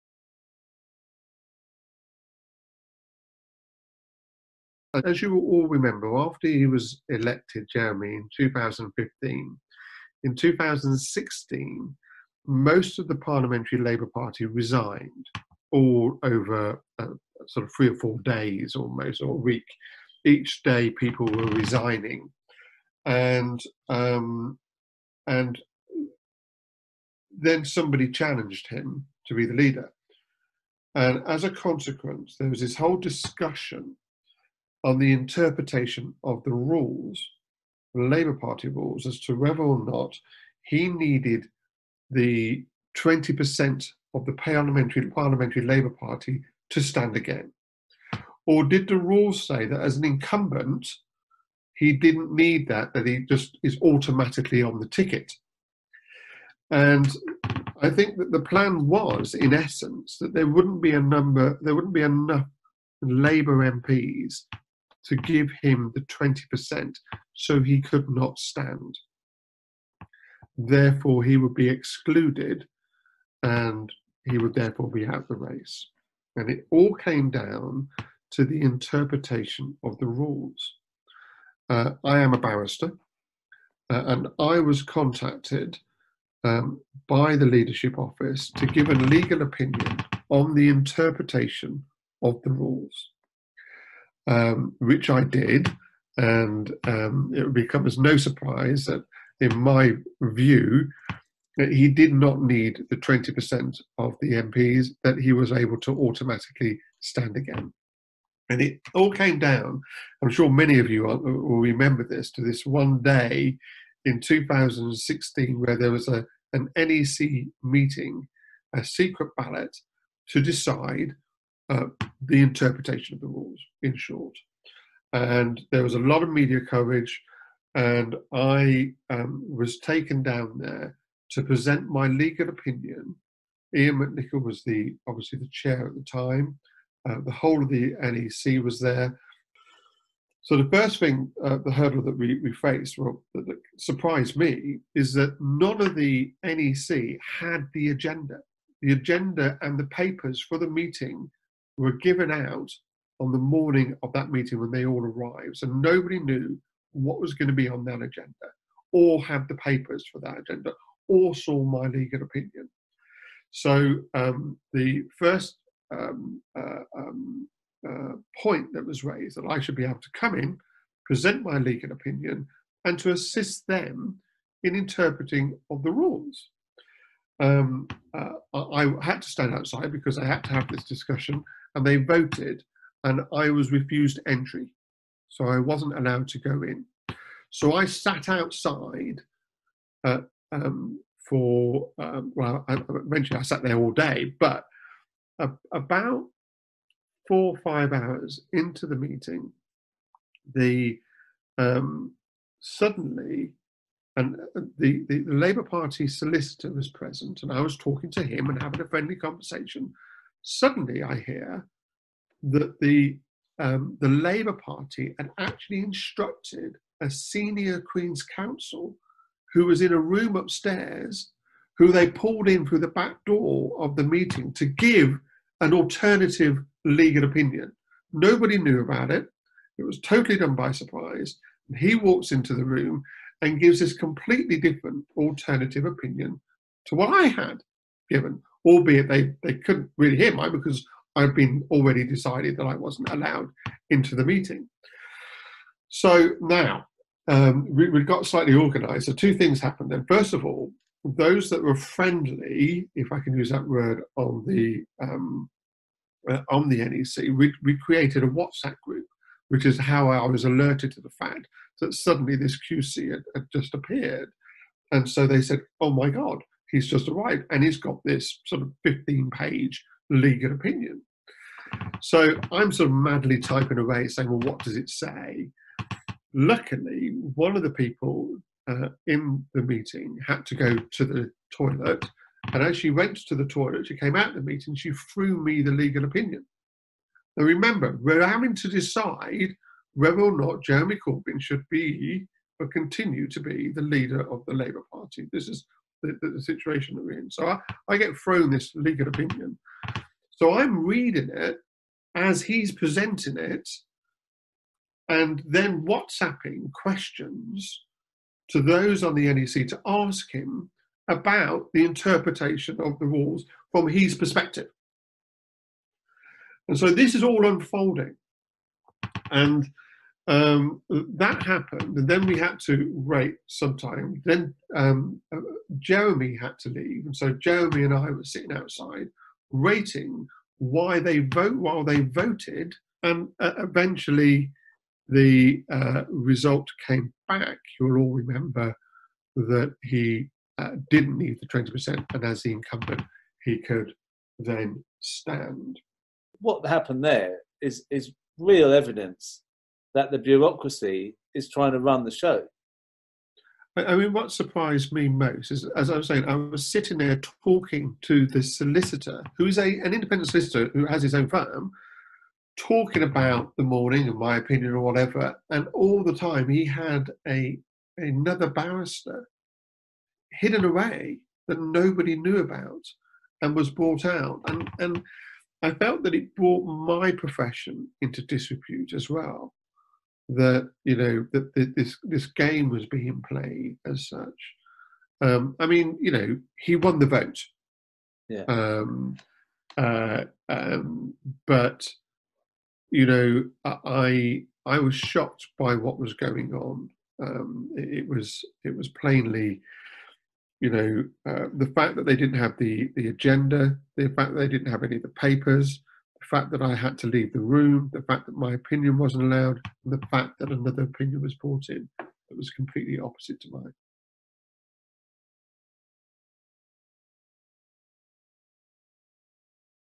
WhatsApp stopped the coup against Corbyn
Mark McDonald recalls the day Labour Party bureaucrats tried to end Jeremy Corbyn’s leadership